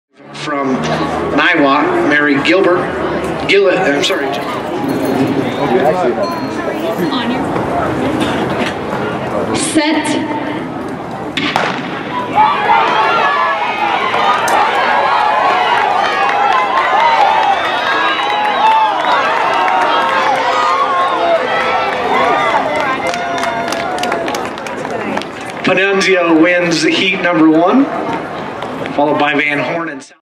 100m Prelims for Colorado State Track Championships 5-17-2018
This video is about 100m Prelims for Colorado State Track Championships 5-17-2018